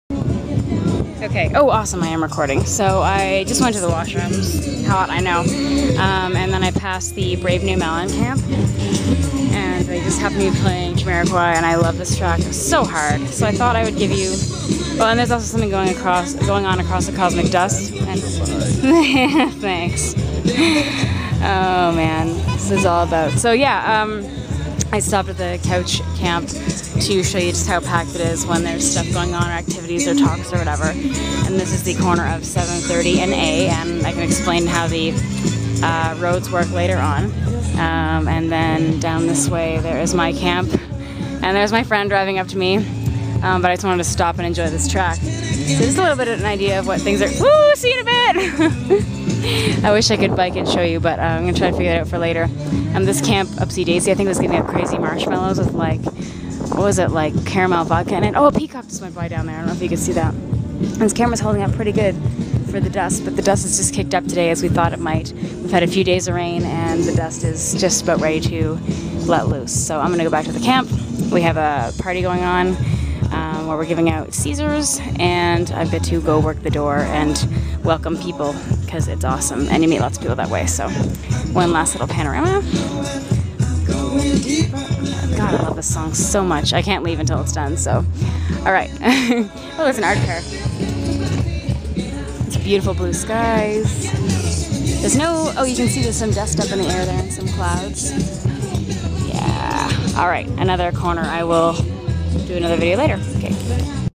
Jay Kay Stops Me At Burning Man (In theory....)
Driving back to my place on the playa, heard this track and I had to stop.. reflect while i was at it. Why not.